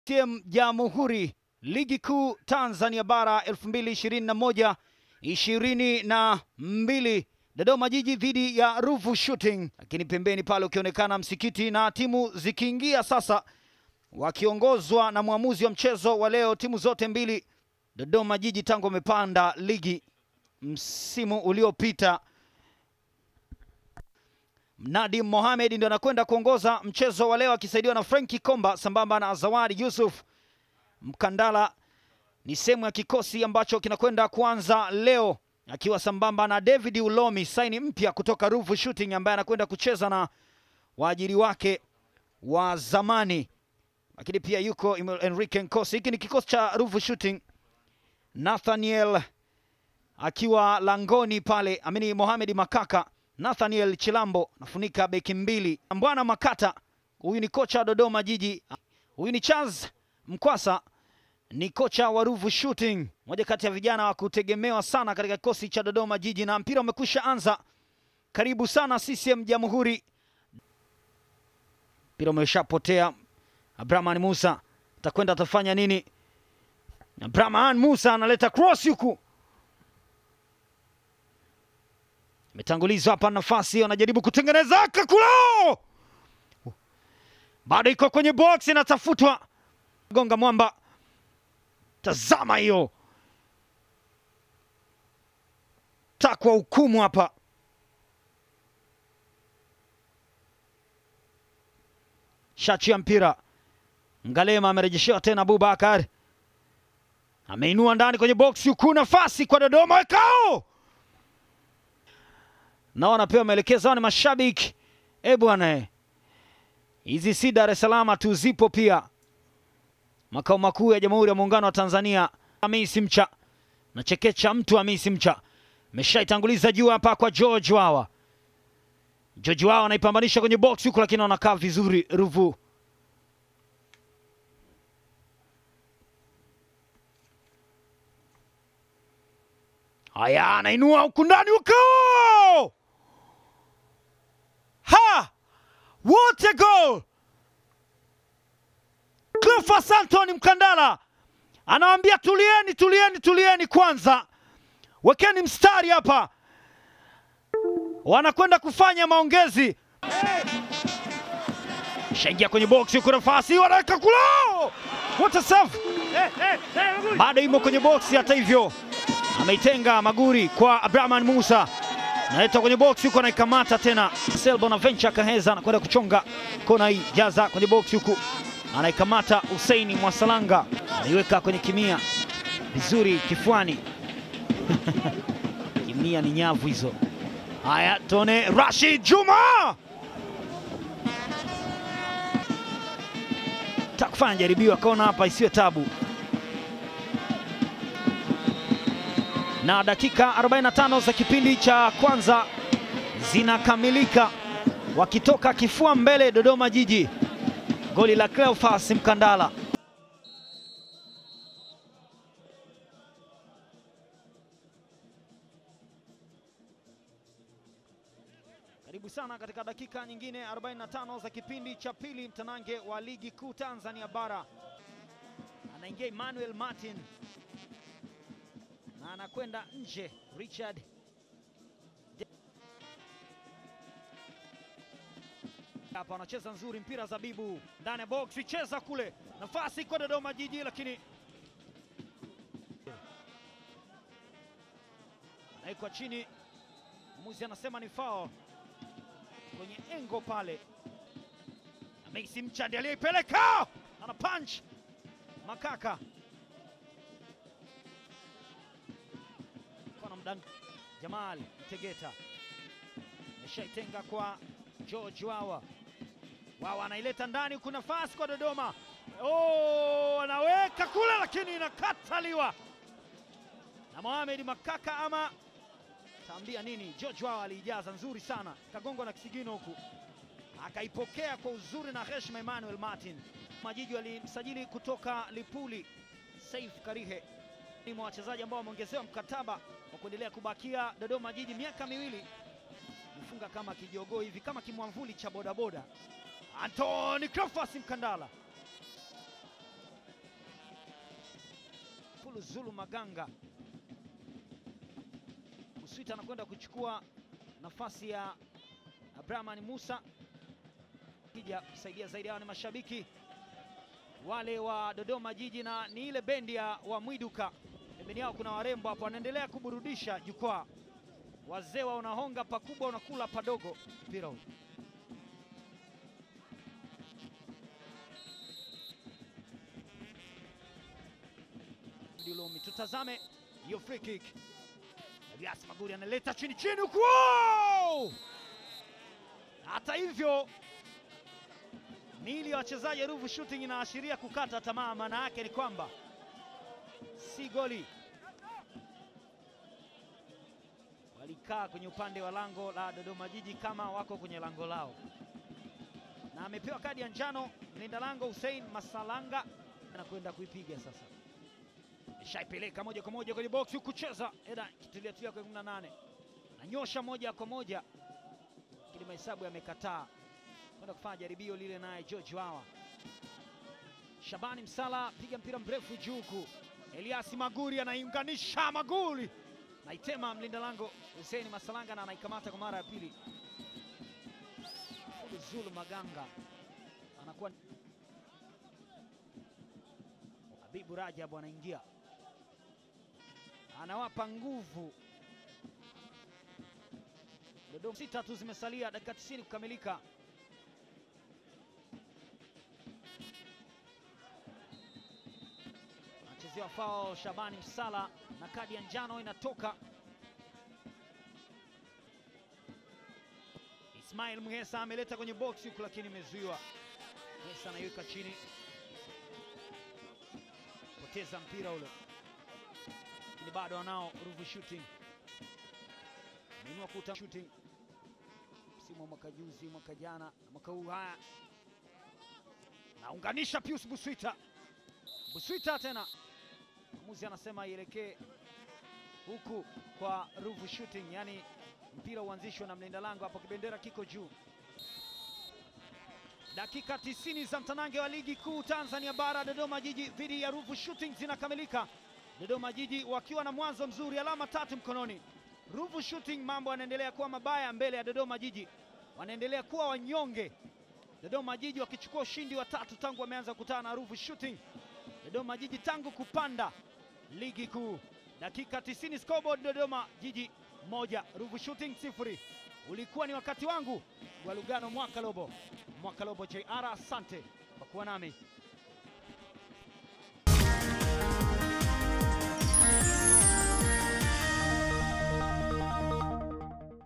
Highlights | Dodoma Jiji 1-0 Ruvu Shooting | TPL 28/09/2021
Goli pekee la Cleophace Mkandala limeipa Dodoma Jiji ushindi wa bao 1-0 dhidi ya Ruvu Shooting, katika mchezo wake wa kwanza wa msimu kwenye Dimba la Jamhuri, Dodoma.